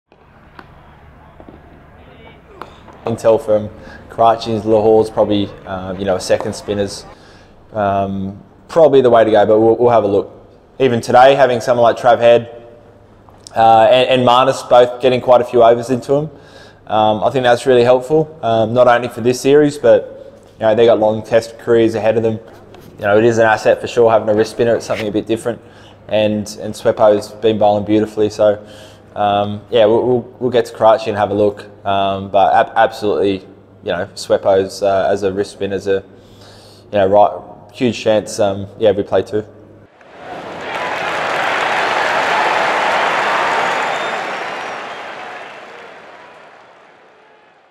Swepson a 'huge chance' as series shifts to Karachi, Lahore | Pakistan v Australia 2022
Queensland leg-spinner Mitchell Swepson could be in line for a long-awaited Test debut with conditions in Australia's next two Tests against Pakistan in Karachi and Lahore expected to favour playing two spinners